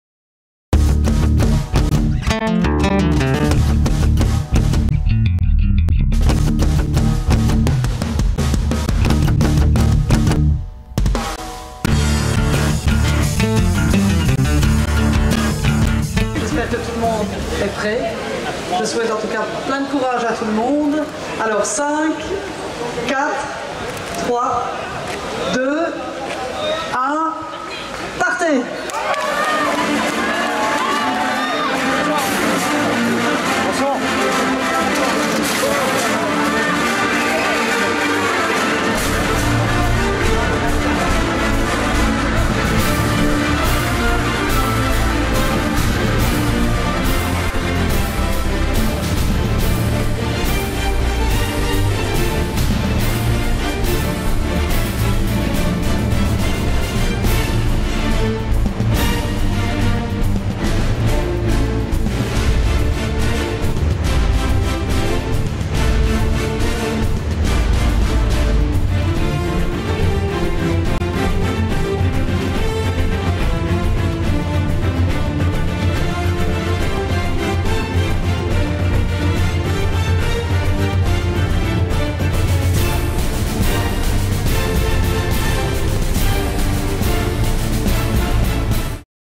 Lancement de la course de garçons de café par Joëlle Milquet
Cent vingt personnes, amateurs et professionnels, ont participé ce dimanche 19 septembre 2010 dans le parc du Cinquantenaire à Bruxelles à la 23e course de garçons de café, organisée dans le cadre de l'opération Pineau sur rue.